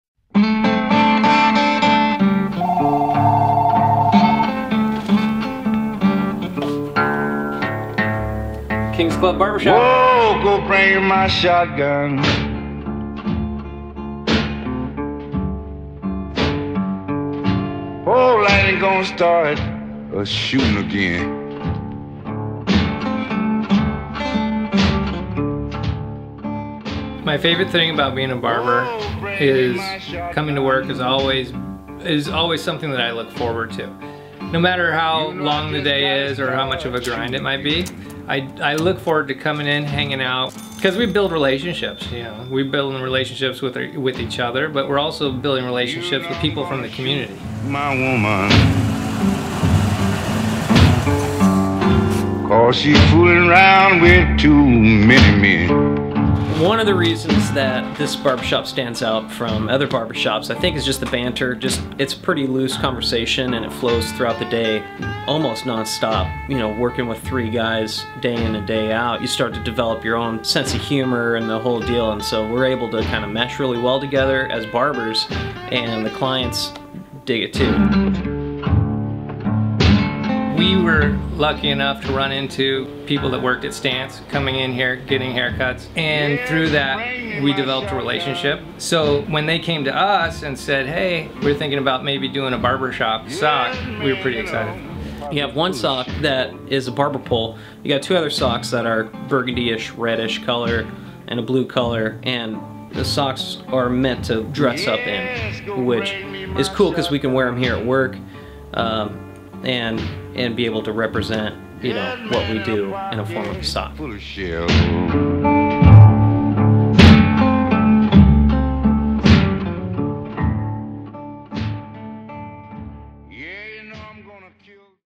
THREADS Ep.04 - Kings Club Barbershop
In Ep.04 of Threads, we teamed up with our favorite barbershop Kings Club Barber Shop in Dana Point to create a collection which offers a step back in time. As scratchy jazz and the sweet aroma of Bay Rum fill the shop, barbers dressed in slacks and ties offer haircuts and shaves with an uncompromising attention to detail.